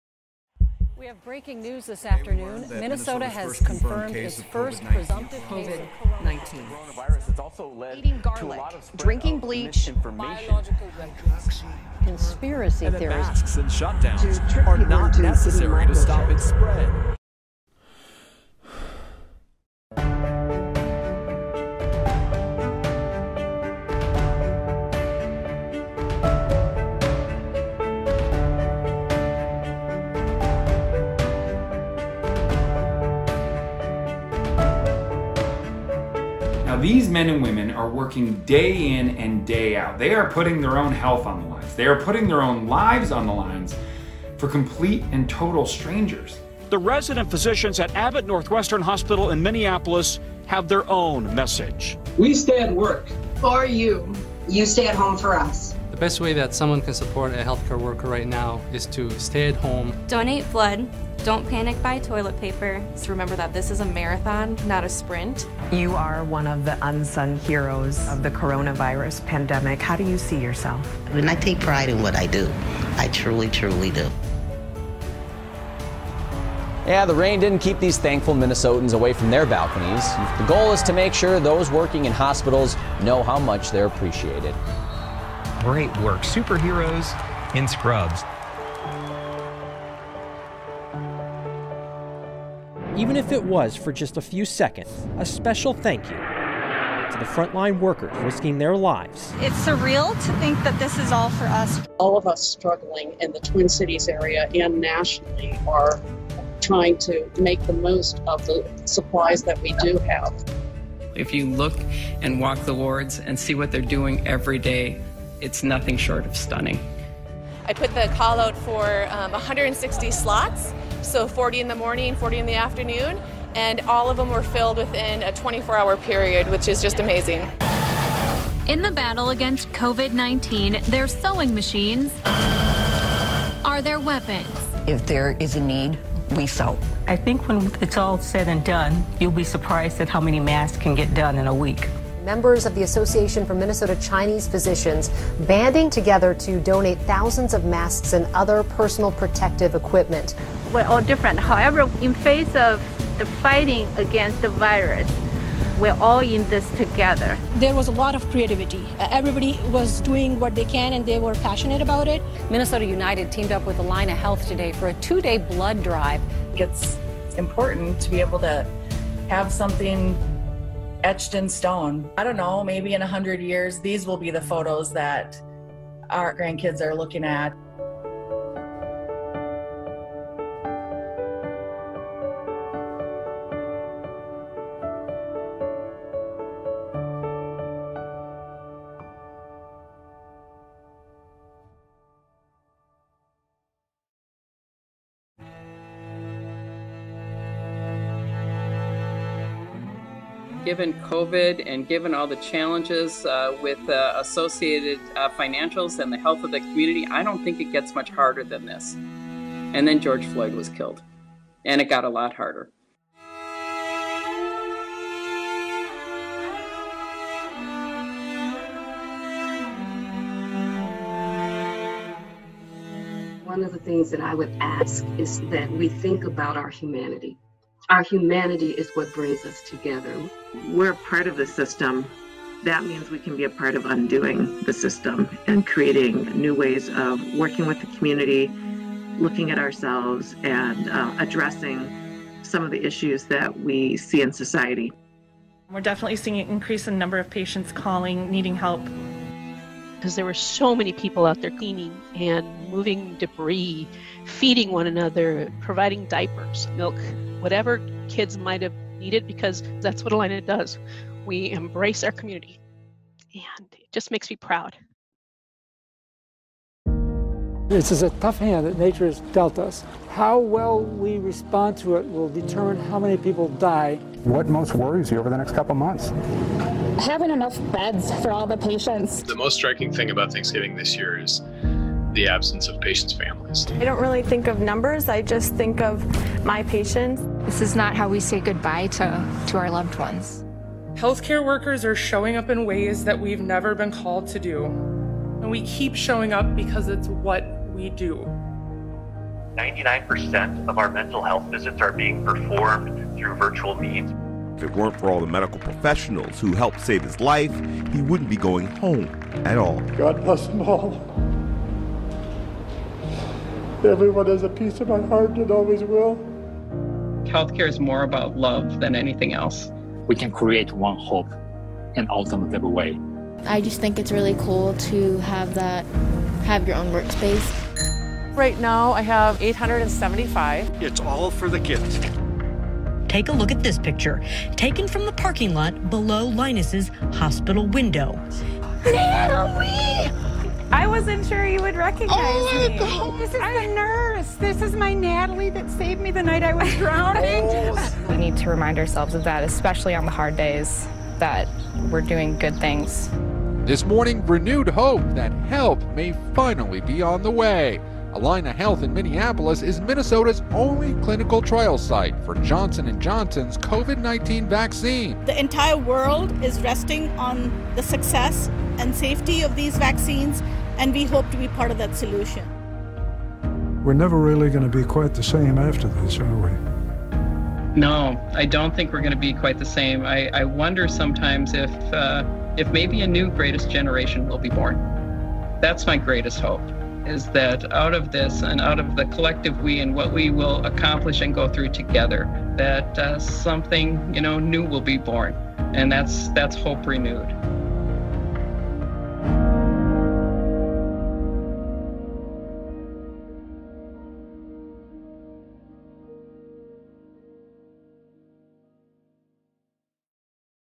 Allina Health 2020 Reflections
At Allina Health, we’ve always known that health care workers are strong, resilient, and compassionate. But the COVID-19 pandemic has demonstrated even more how our caregivers are heroes. It’s been one year since COVID-19 began circulating in Minnesota.

Take a look back with us on all we have overcome and achieved with your help as a community in the past year, and learn more about our Caring for Caregivers fund, which provides immediate financial support for our employees who experience financial challenges due to COVID-19.